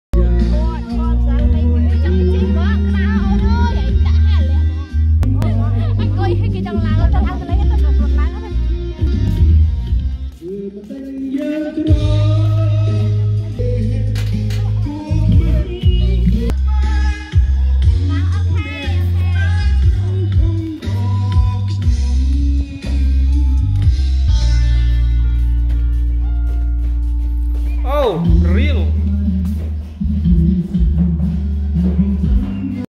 Airplan on the ground ✈️
Talo Sen Chey Resort. located in Talo Commune, Bakan district, Pursat province, Cambodia.